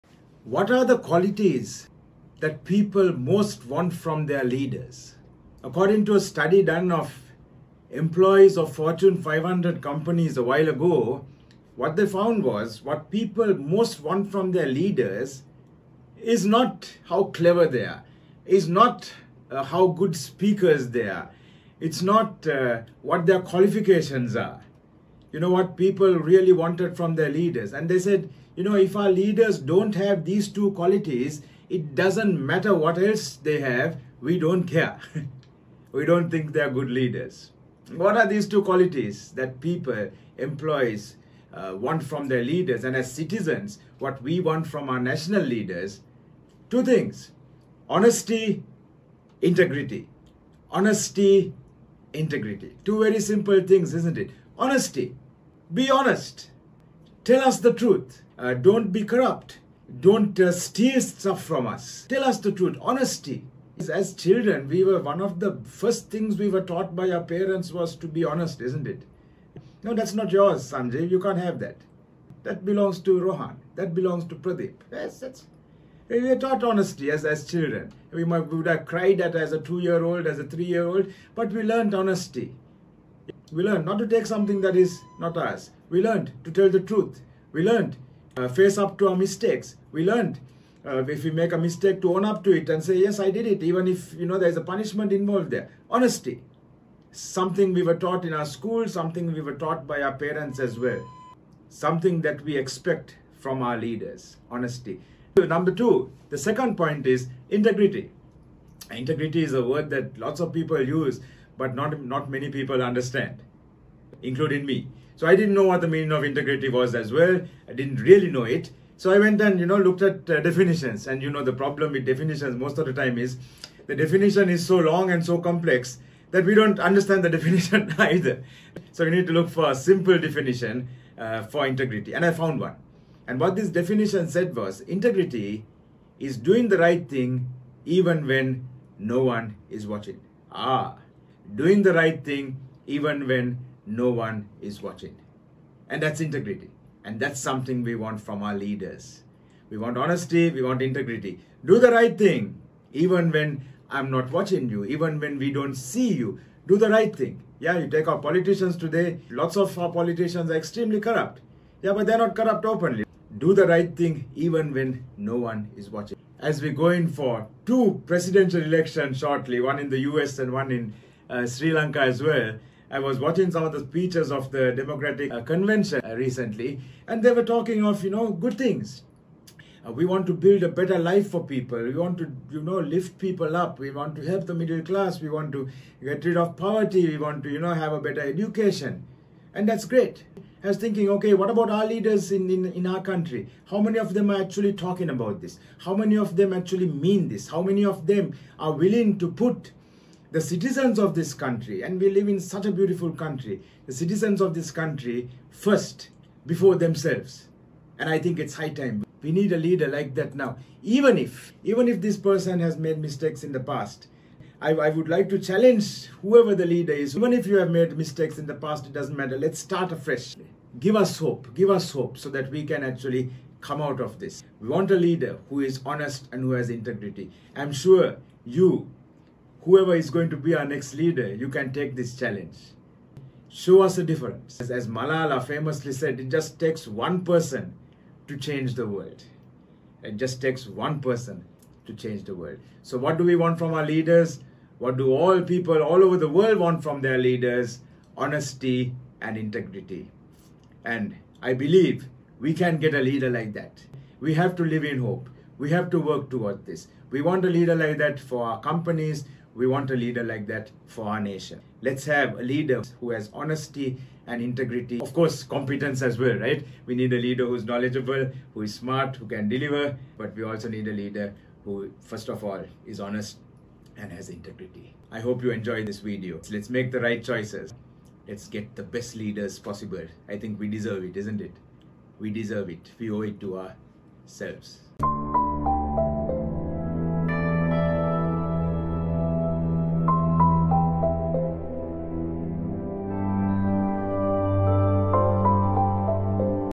2 Things That Matter the Most in Leadership | Learning with Sanjeev Jayaratnam
👐 2 Things That Matter the Most in Leadership 👐

Discover why honesty and integrity are crucial qualities people seek in their leaders. In this insightful discussion, Sanjeev Jayaratnam explores the core values that define effective leadership and how these traits can transform organizations and nations.

👉 Practical Tips:

* Embrace Honesty: Always be truthful and transparent in your communication.
* Show Integrity: Do the right thing even when no one is watching.
* Lead by Example: Set a standard for others to follow by demonstrating these qualities consistently.

🔍 Key Takeaways:

* Honesty Builds Trust: Leaders who are honest gain the trust and respect of their followers.
* Integrity Drives Excellence: Acting with integrity ensures long-term success and ethical behavior.
* Inspire Change: Effective leaders inspire others to uphold these values and lead by example.

📚 Chapters:

0:00 - Introduction
0:58 - Honesty
1:48 - Integrity
2:45 - Real-Life Examples and Challenges
4:02 - Conclusion

Join us to understand the essential qualities that make a great leader and why they matter more than any qualifications or skills.

13 People Skills That You Need to Have Be a Great Leader| Learning with Sanjeev Jayaratnam